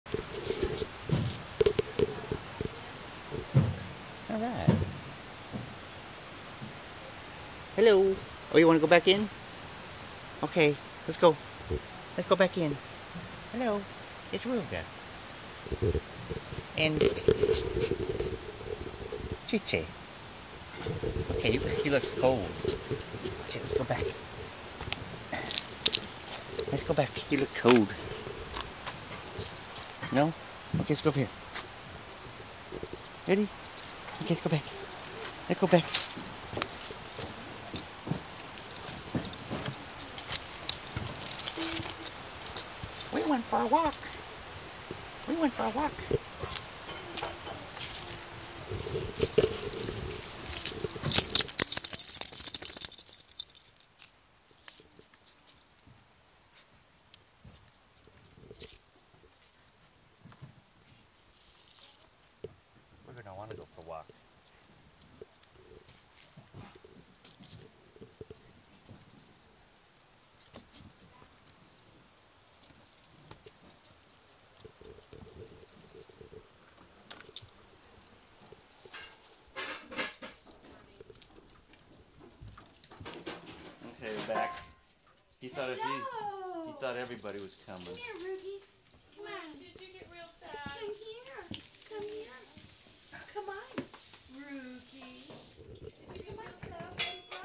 "where do you want to go ?" I dunno where do you want to go ? I dunno........
Cheechi and Rugar almost went for a walk in El Portal